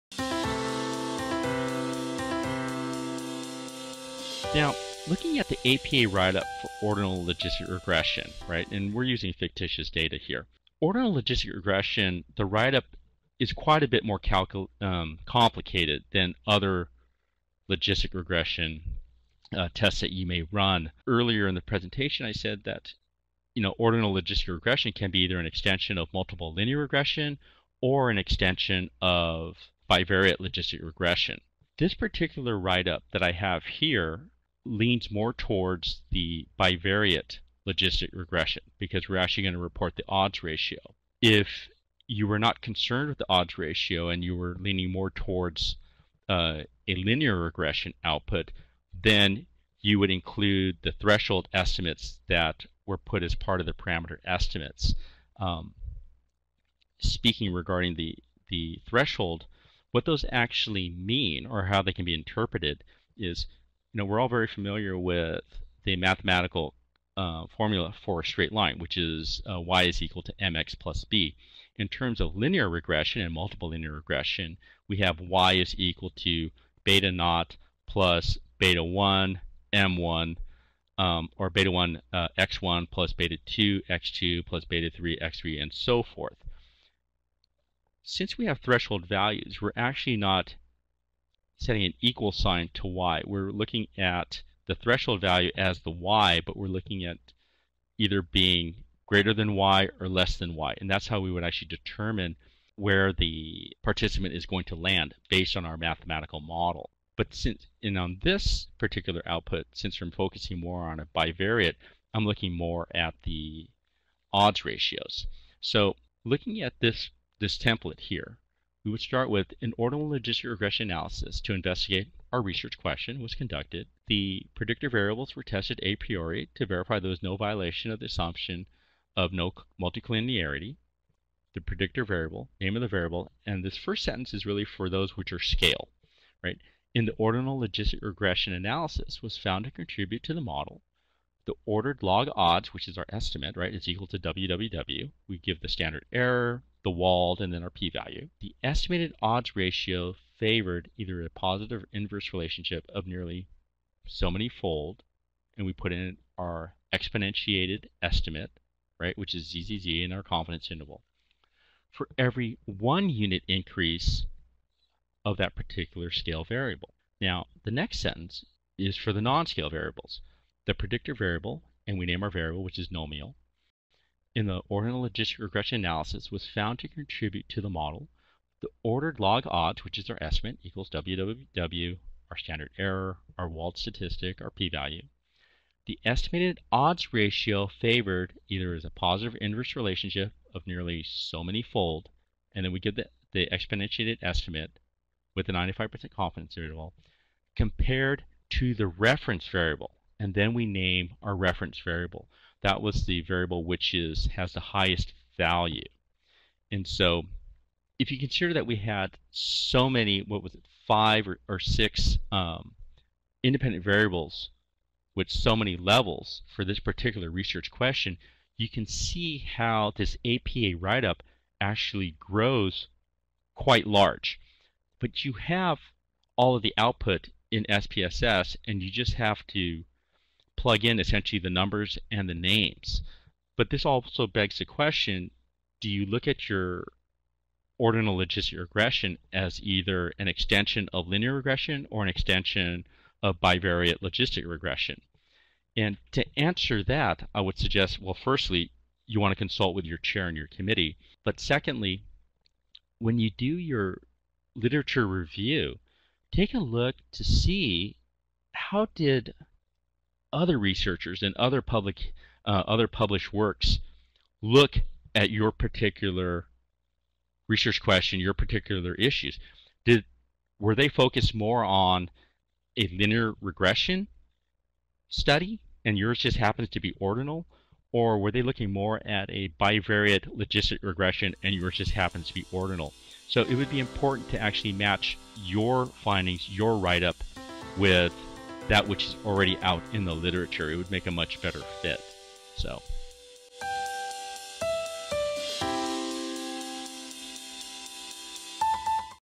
Ordinal Logistic Regression - APA Write-Up
Join former statistics tutor and Walden University graduate, Dr. Zin Htway, for an introduction to the statistical test, Ordinal Logistic Regression.

This video was edited for quality and length by Shawn Picht and Shawna Burtis, PhD.

*Originally recorded for Walden University's Academic Skills Center in March 2016 by as a live webinar by Dr. Zin Htway.